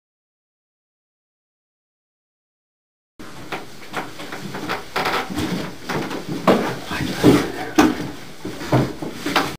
A Ghostly EVP Captured During Paranormal Investigation
Here is the first of three EVPs we captured during a recent Private Business investigation in Springfield, Ohio. This is the same investigation where we captured the video of a possible Spirit communicating through at attic light. Let us know what you think of the EVP.